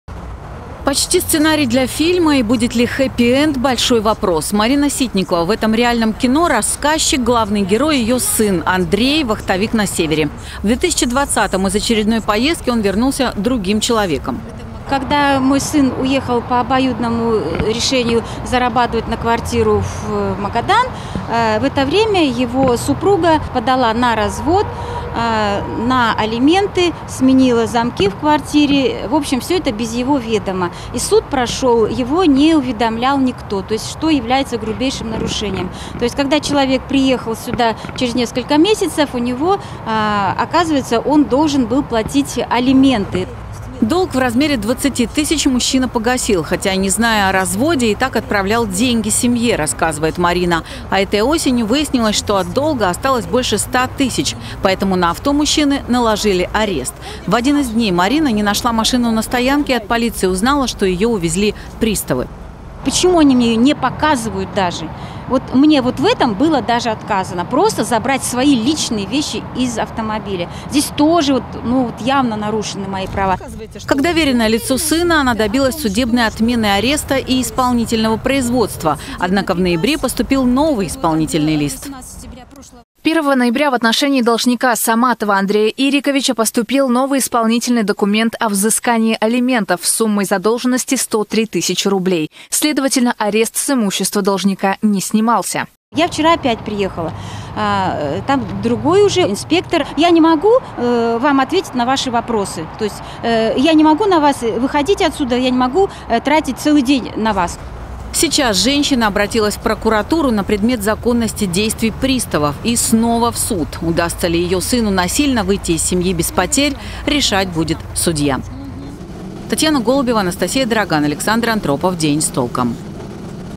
Барнаулец, вернувшись с вахты, узнал, что разведен, не платит алименты и лишается имущества
Уезжал на вахту счастливым отцом семейства, а позже узнал, что разведен, не платит алименты и лишается имущества. В такую историю попал барнаулец Андрей Саматов. Сам он даже не может себя защищать, так как почти круглый год находится на заработках на Севере, а его представителя в органах не принимают